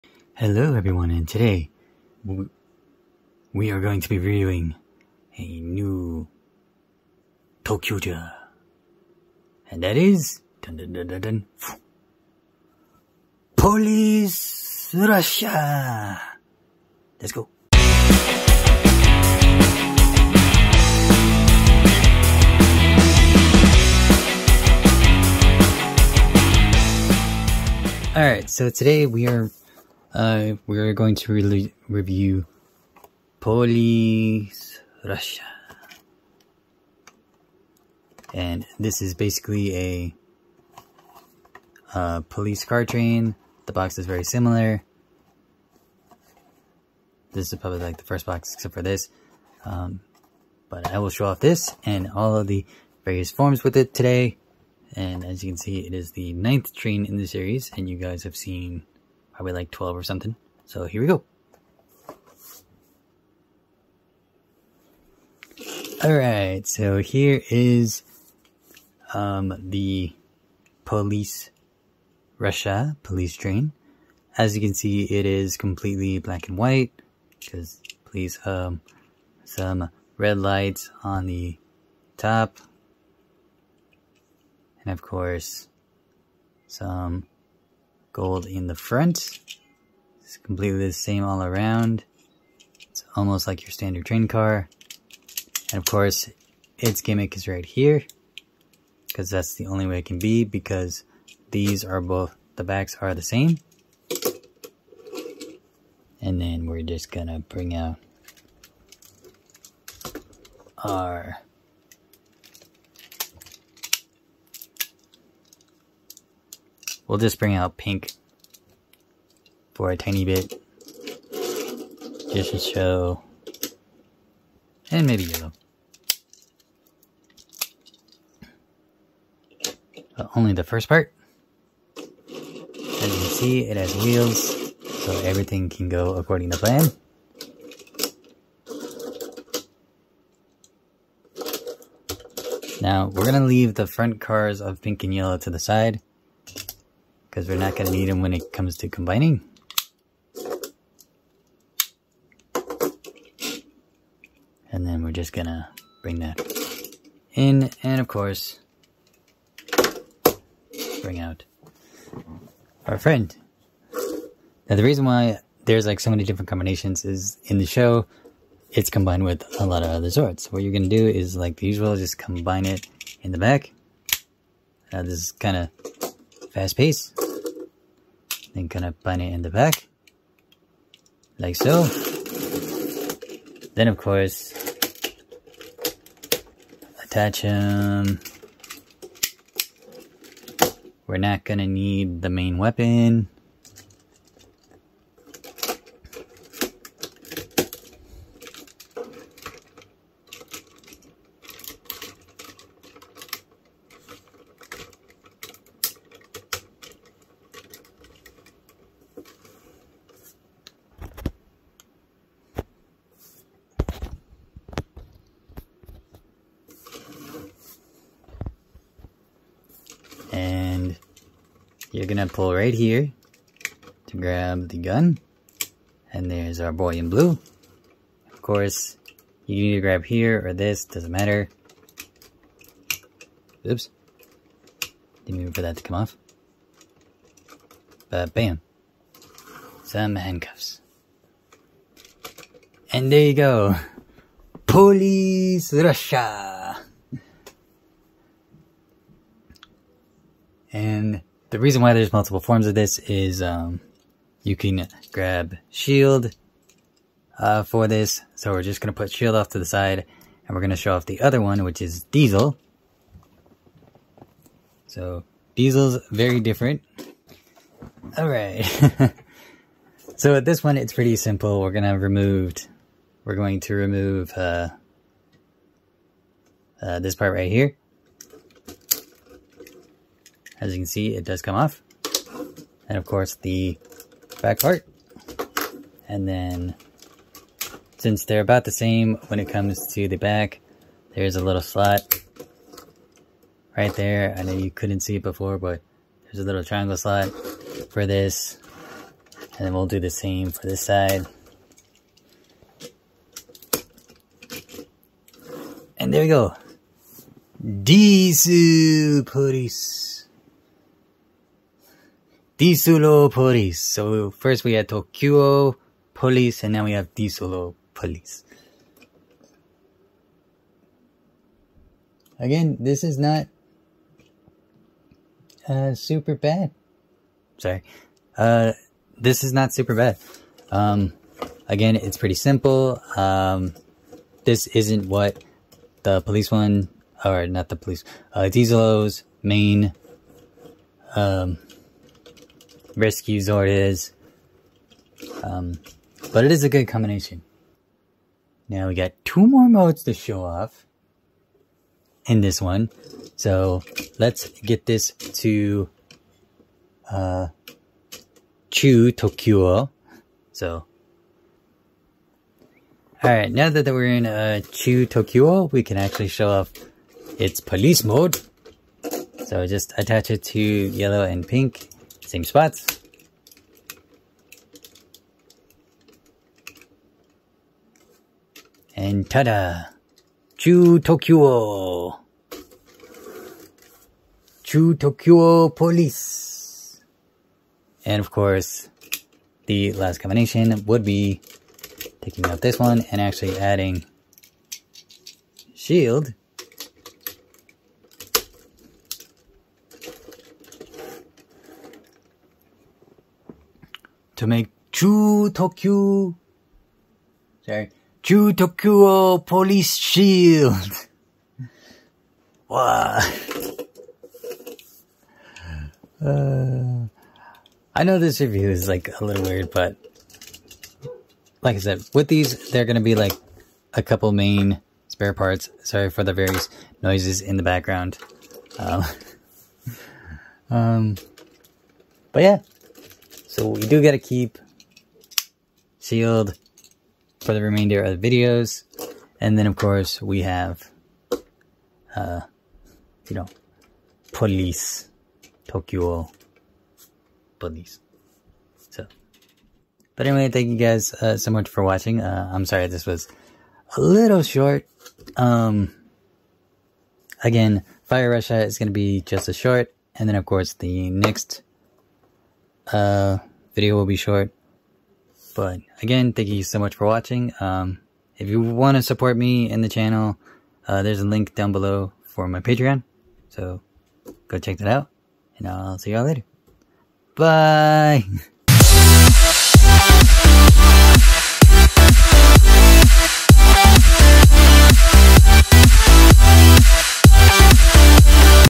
(Ressha Sentai ToQger) Review of the Police Ressha Zord
Welcome to Sabastion's ToyCon. There will be various toys besides zords and transformers. I get more reviews when I get my hands on more toys.

*This video is for 14 age and older